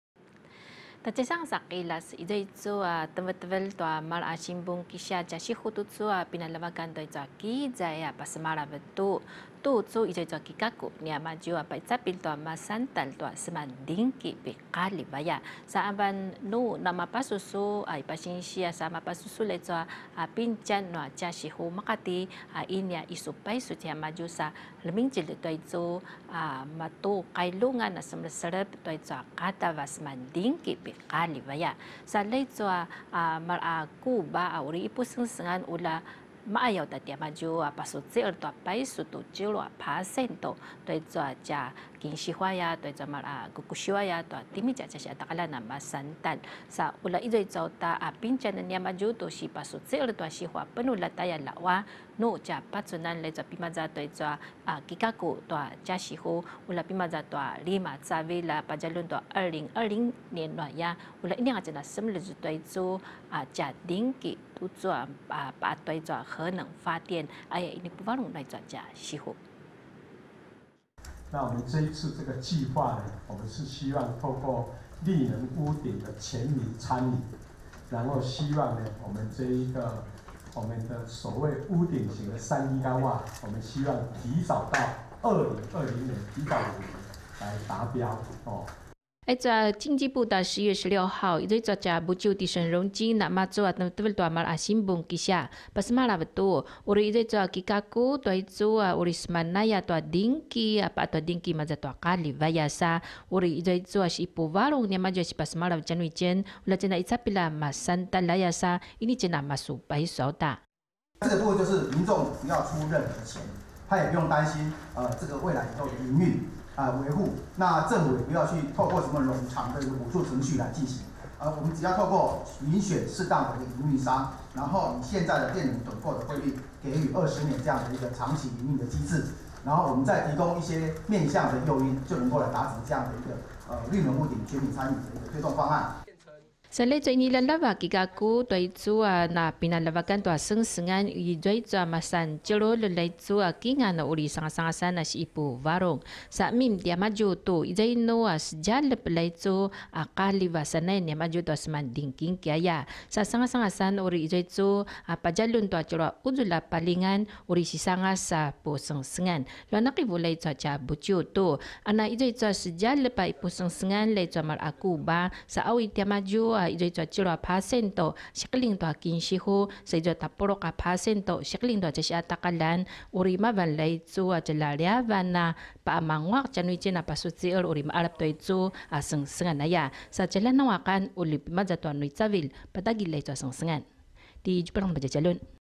"綠能屋頂.全民參與" 計畫最快107年上路 2017-12-06 Paiwan TITV 原視族語新聞
ulja 'icapilj itjen a masantalj tua semandinki piqaljiv

經濟部11月16日在部長沈榮津的主持下，召開記者會，公布「綠能屋頂 全民參與」的計畫，計畫中推出許多的誘因，鼓勵民眾投入公民電廠，其中一項就是民眾完全不用出資。

能源局預計有3縣市優先投入進行，先盤點具潛力的綠能屋頂，希望在第一階段就能夠達到3000戶來做示範驗證。

經濟部長沈榮津強調，符合資格的營運商，則必須回饋地方政府3%，以及當地民眾至少10%的收益 ，並由提供最優惠回饋者得標，能源局希望最快在107年正式上路。